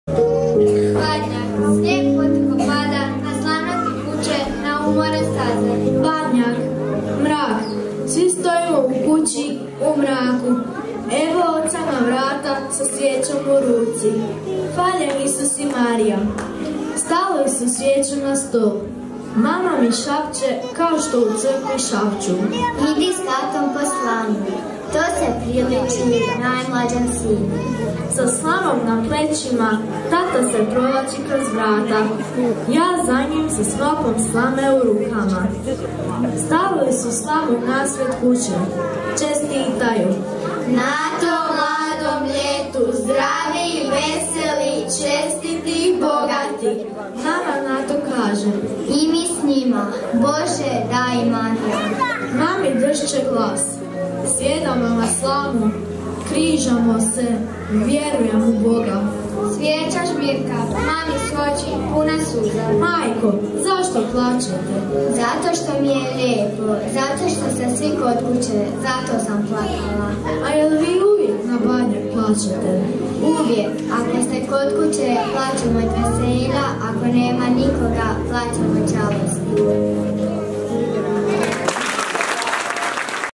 Badnjak
OŠ "August Cesarec" Ivankovo

Priredba povodom Božića 2016. godine.